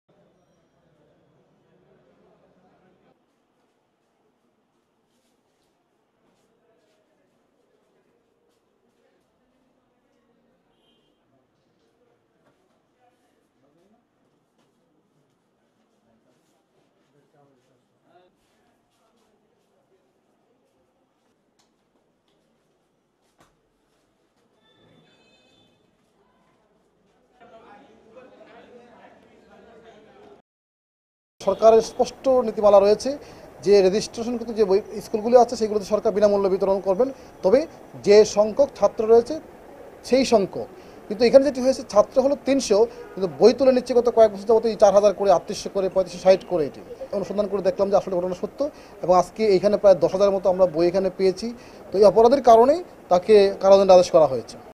১০ হাজার সরকারি বই জব্দ করেছে র‍্যাব | Somoy TV News
রাজধানীর গোড়ান আদর্শ বিদ্যালয় থেকে অন্তত ১০ হাজার সরকারি বই জব্দ করেছে র‌্যাবের ভ্রাম্যমাণ আদালত। এ ঘটনার সঙ্গে জড়িত থাকার অভিযোগে বিদ্যালয়ের প্রধান শিক্ষককে ৩ মাসের বিনাশ্রম কারাদণ্ড দিয়েছেন আদালত।